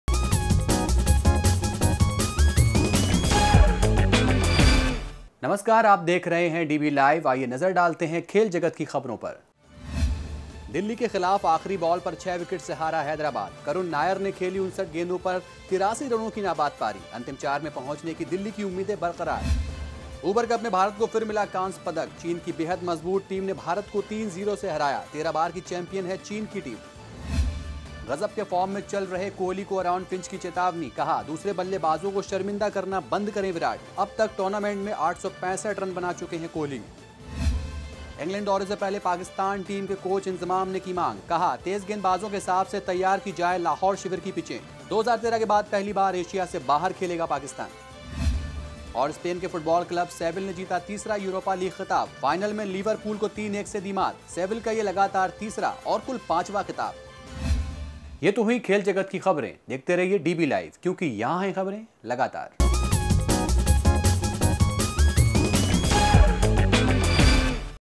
DBLIVE | 21 May | Sports News Headlines
1- दिल्ली के खिलाफ आखिरी बॉल पर छह विकेट से हारा हैदराबाद...करुण नायर ने खेली 59 गेंदों पर 83 रनों की नाबाद पारी...अंतिम चार में पहुंचने की दिल्ली की उम्मीदें बरकरार
2- उबर कप में भारत को फिर मिला कांस्य पदक…चीन की बेहद मज़बूत टीम ने भारत को 3-0 से हराया...13 बार की चैम्पियन है चीन की टीम
3- गजब के फार्म में चल रहे कोहली को एरोन फिंच की चेतावनी, कहा, दूसरे बल्लेबाजों को शर्मिंदा करना बंद करें विराट...अबतक टूर्नामेंट में 865 रन बना चुके हैं कोहली
4- इंग्लैंड दौरे से पहले पाकिस्तान टीम के कोच इंजमाम ने की मांग...कहा, तेज गेंदबाजों के हिसाब से तैयार की जाएं लाहौर शिविर के लिए पिचें...2013 के बाद पहली बार एशिया से बाहर खेलेगा पाकिस्तान
5- स्पेन के फुटबॉल क्लब सेविल ने जीता तीसरा यूरोपा लीग खिताब... फाइनल में लिवरपूल को 3-1 से दी मात... सेविल का यह लगातार तीसरा और कुल पांचवां खिताब